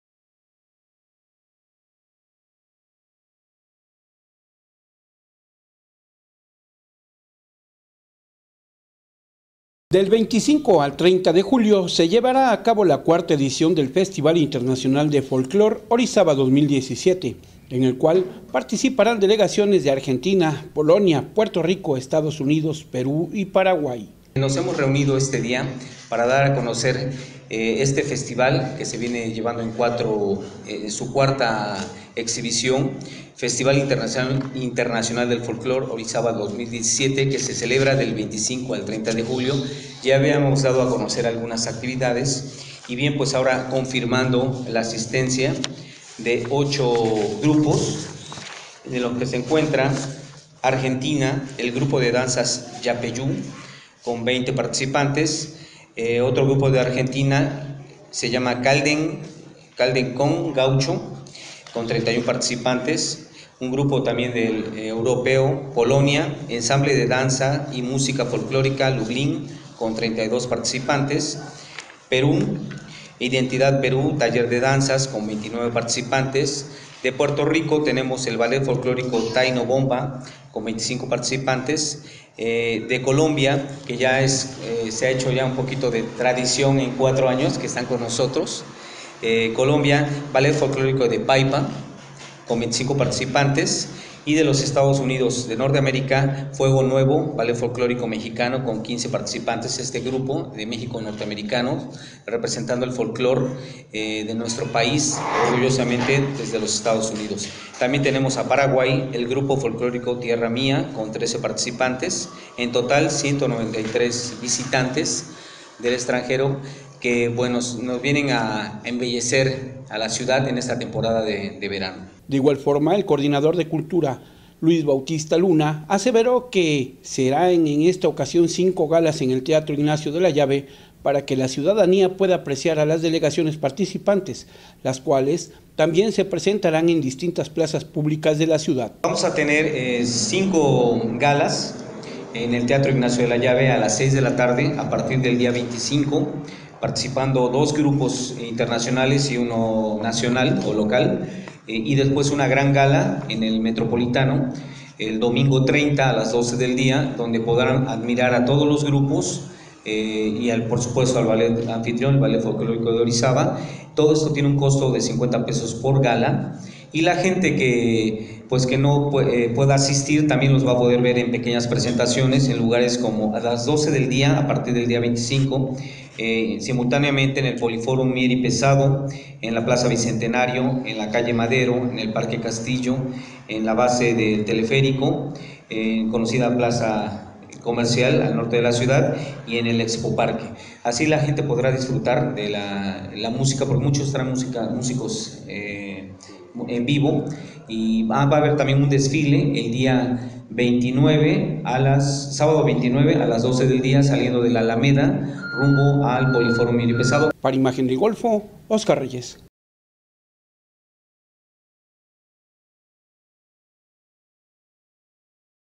4° Festival Internacional de Folklore del 25 al 30 de julio
Este año serán 5 galas las que se presentarán